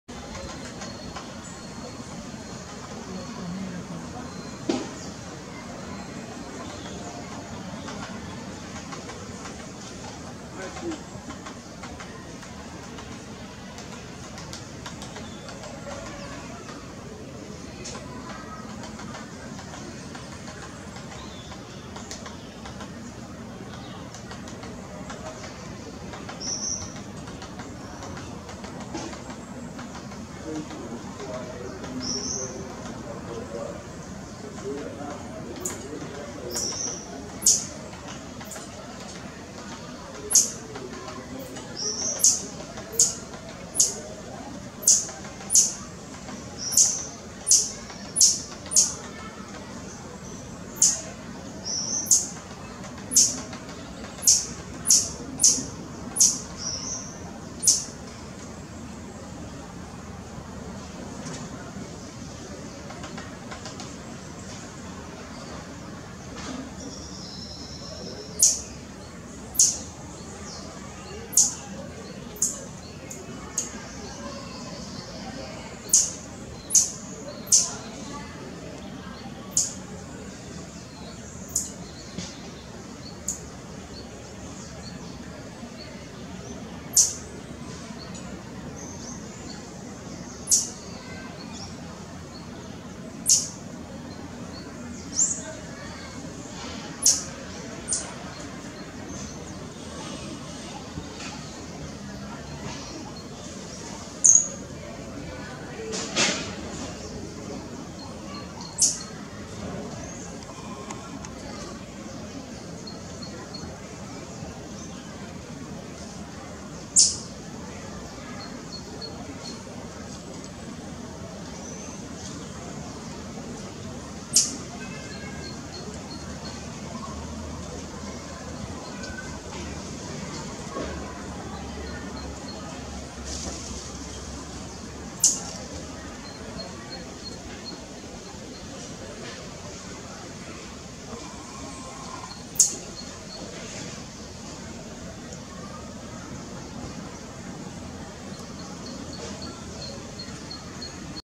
pica pau anão barrado
Rio de Janeiro (Parque da Chacrinha), agosto de 2018.
Picumnus cirratus